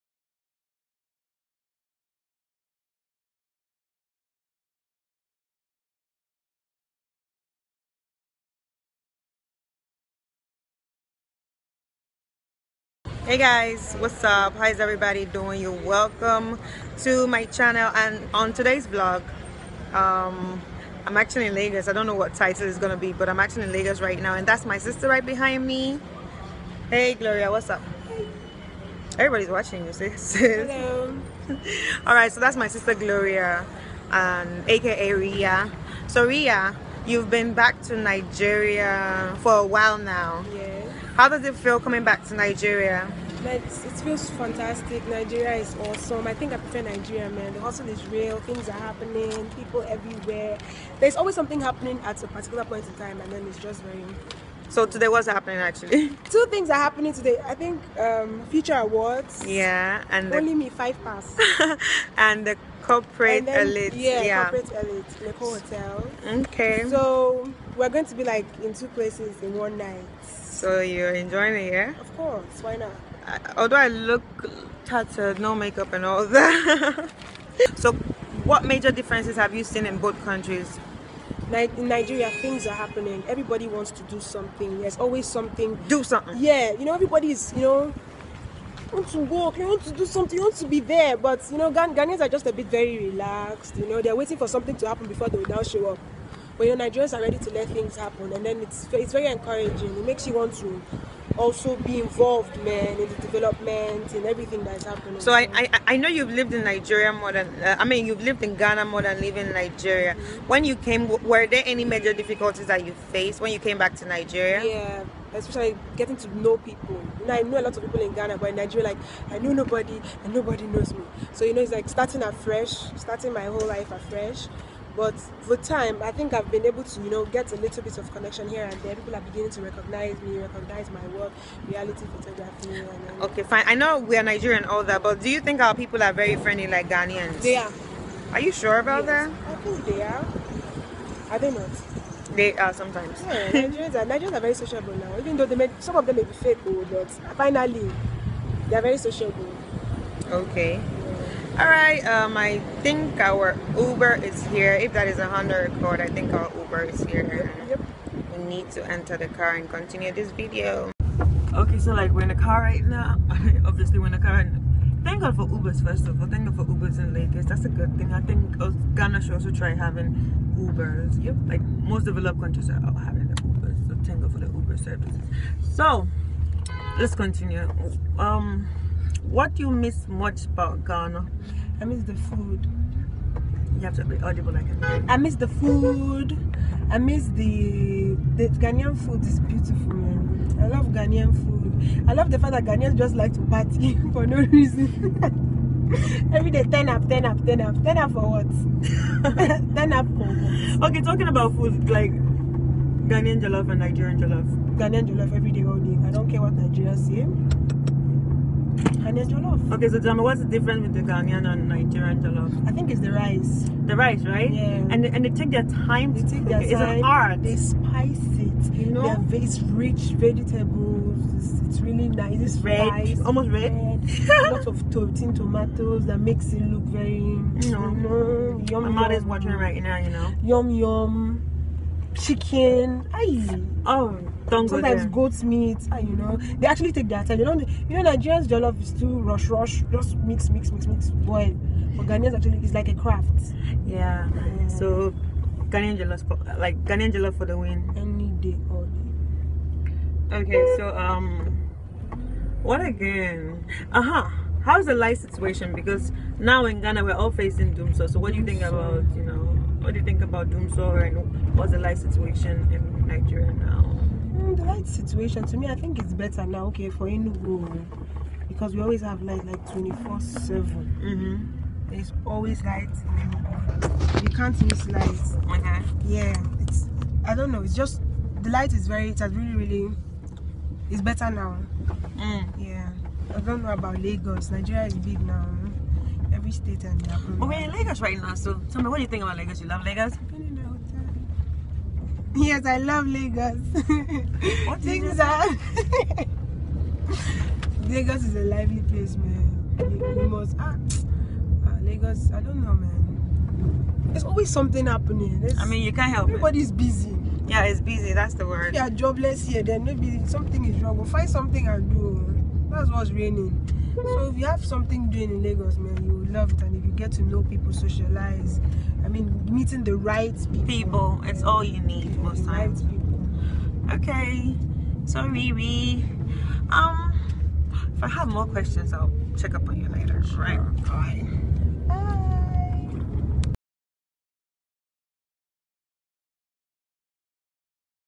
Little Differences From Ghana And Nigeria
Visited Nigeria, and met my little sister who has lived in Ghana most of her live but recently moved back to Nigeria...